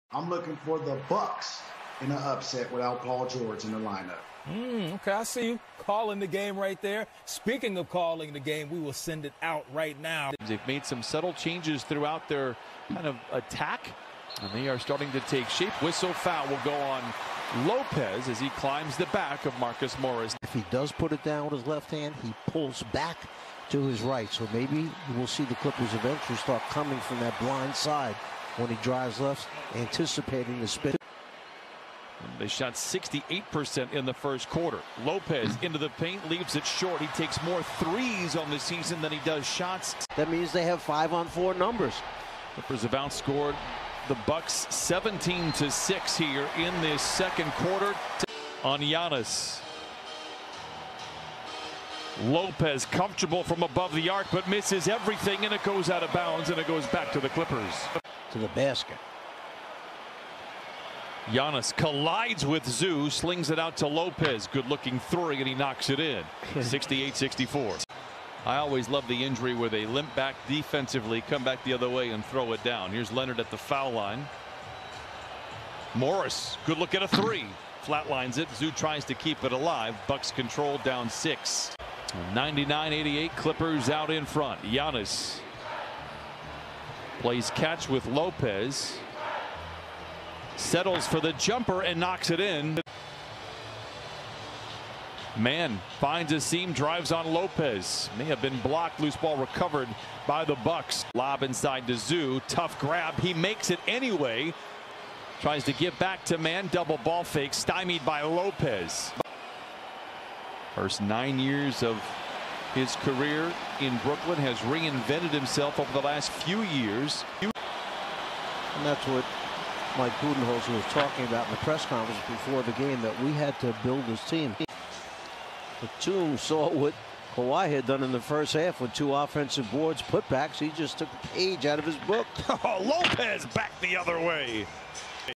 Brook Lopez 7 PTS: All Possessions (2021-03-29)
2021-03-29 Los Angeles Clippers vs Milwaukee Bucks: Player Highlights Brook Lopez
    7 PTS, 1 REB, 1 AST, 0 STL.
        1 of 2 from 3.
        2 of 4 FGs in 21:39 minutes.